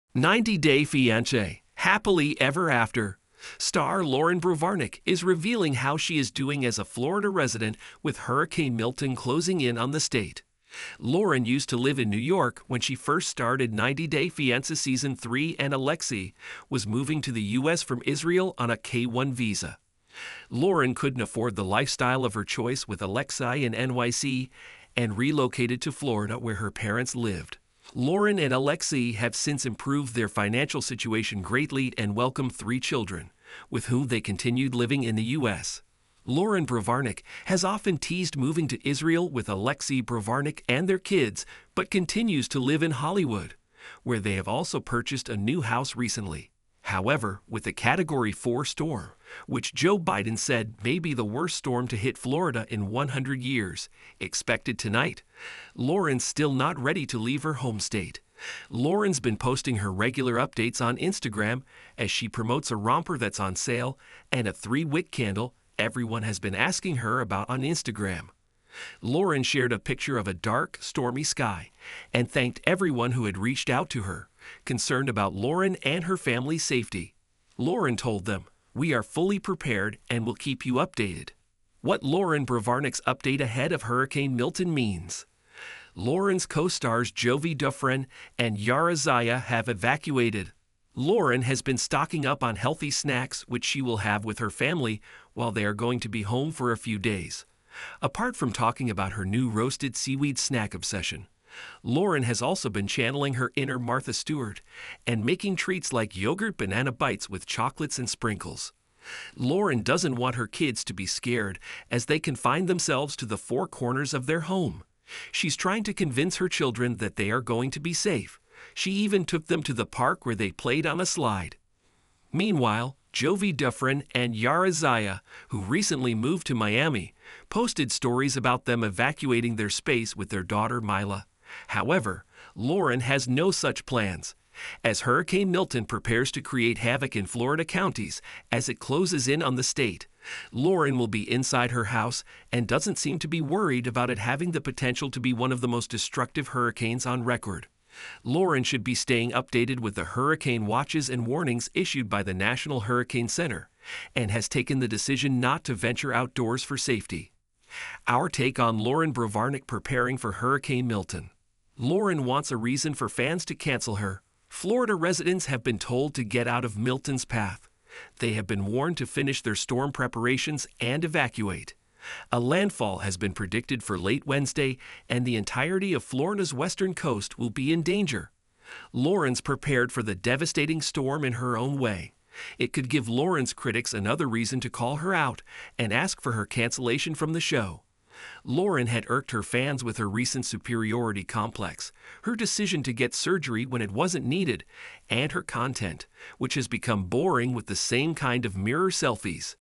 90 Day Fiancé's Loren & Alexei EXPOSE Their Safety Plans as Hurricane Milton Approaches Florida!
🌪️🌴 As Hurricane Milton approaches Florida, 90 Day Fiancé's favorite couple, Loren & Alexei, are taking no chances! In this must-watch video, they reveal their personal safety plans and preparations to weather the storm. From securing their home to stocking up on supplies, these fan favorites share invaluable tips on hurricane readiness. Join them as they educate and inspire their fans to stay safe during this unpredictable season! 💖🏠